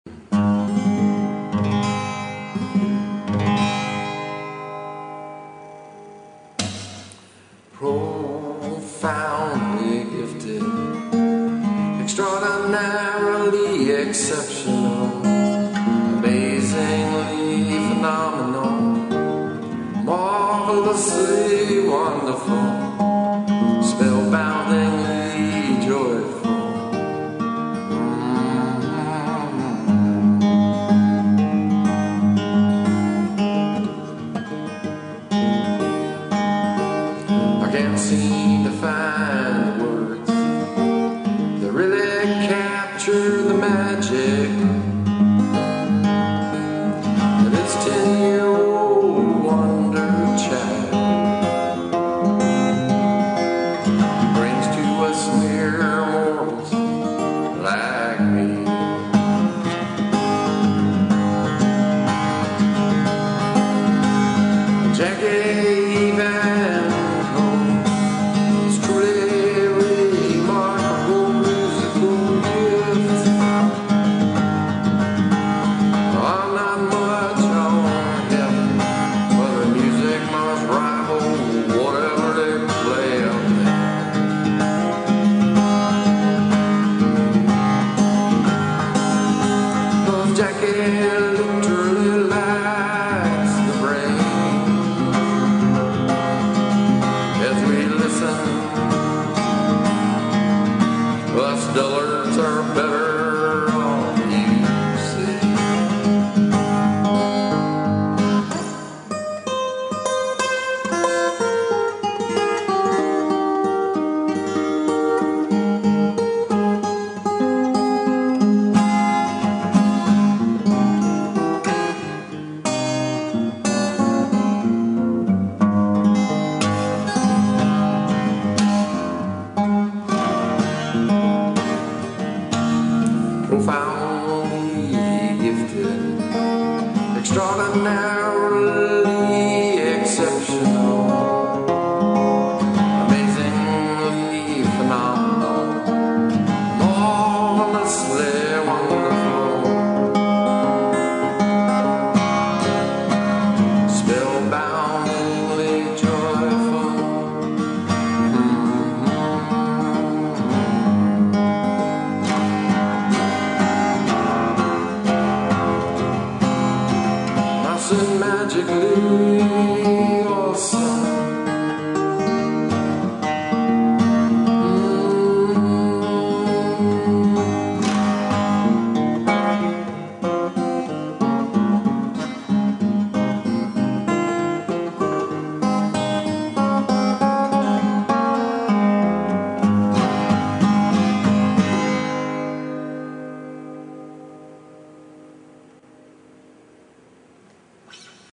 A Ten (10) Year Old Wonder Child (Jackie Evancho)
There are all sorts of musical marvels in this world. There are greats in every niche and they can take you to all sorts of levels. But this 'Ten (10) Year Old Wonder Child) is so amazing, it makes me wonder if we're in for a Golden Age like we haven't seen since Bach, Mozart, & Beethoven were lighting the way. Maybe there are a bunch of these wonder children, but if not, Jackie will light the way. To my knowledge, she's just the best that's ever happen.